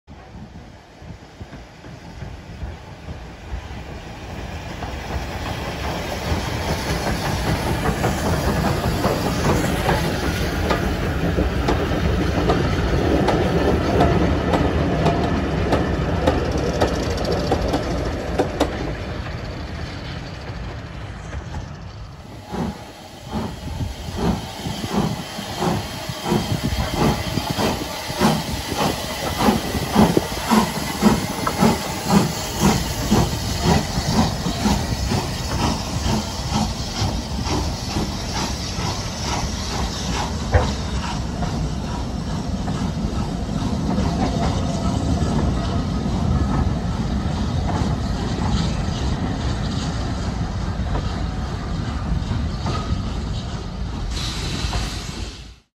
North Norfolk Railway GER Y14 564 Spring Gala April 2022
Short clips of GER Y14 564 from the April 2022 Spring Gala